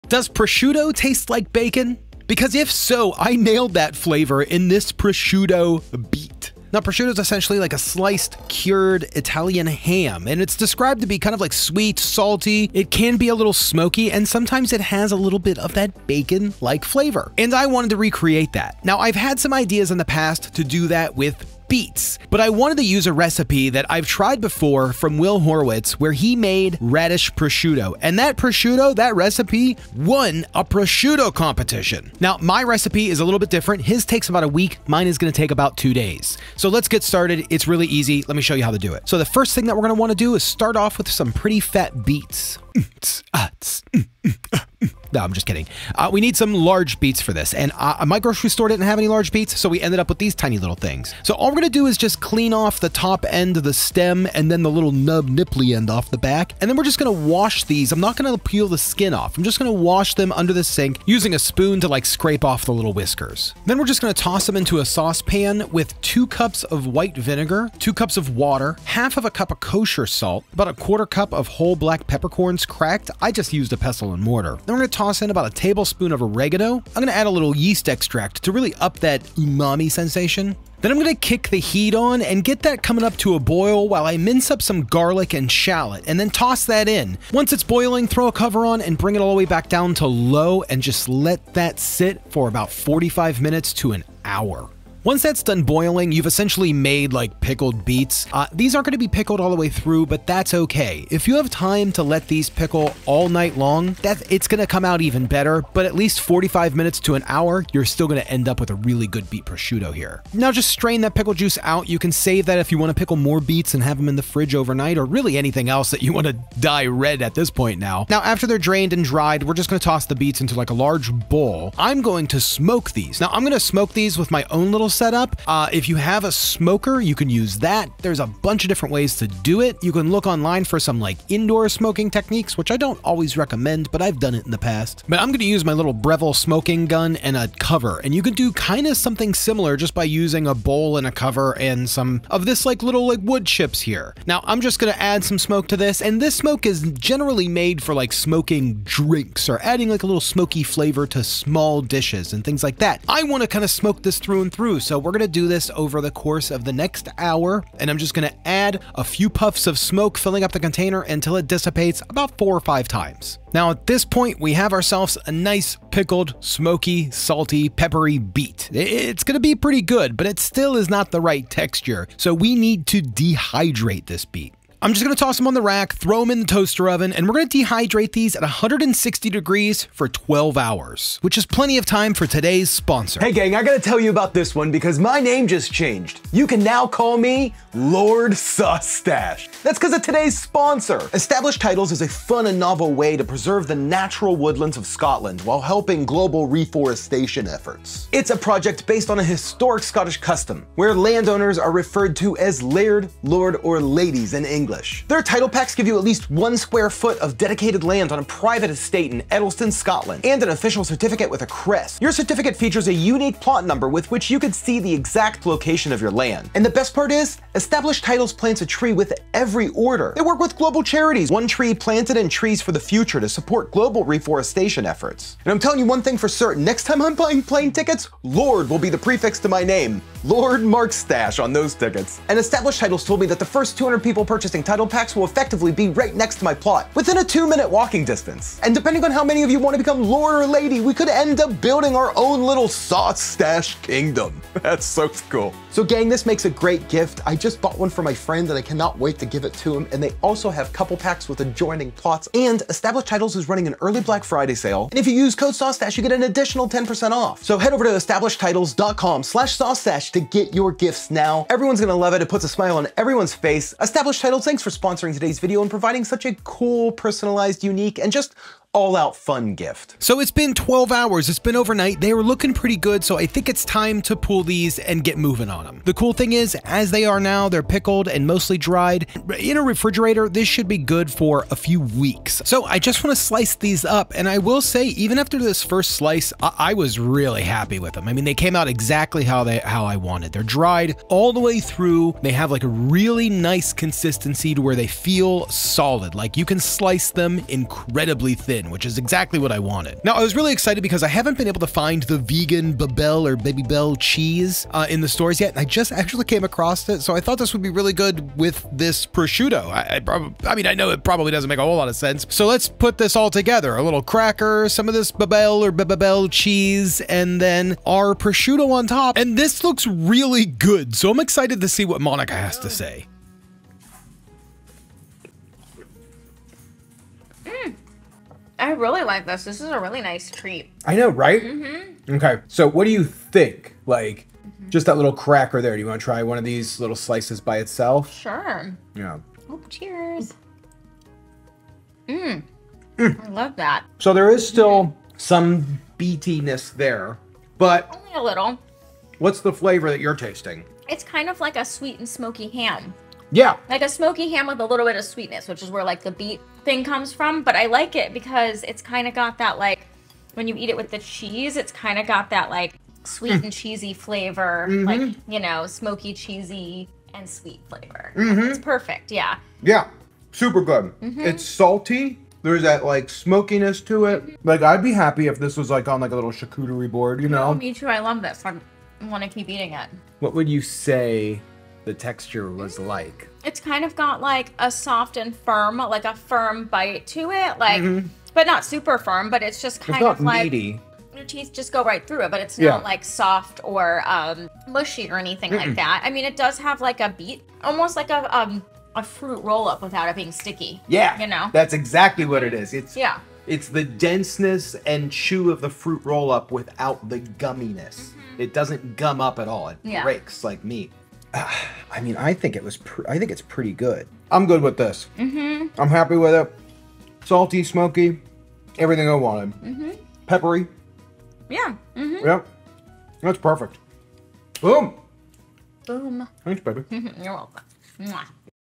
Beet Meat is now a thing You NEED to try
Beet Prosciutto is now a thing you should try... really! Its seriously good and has all the elements I want out of a cured meat... except its made from beets!
I based it off this recipe How to Make Radish Prosciutto with Will Horowitz
Ahh I am so excited for you all to try this!!

By my Book
(Update coming soon)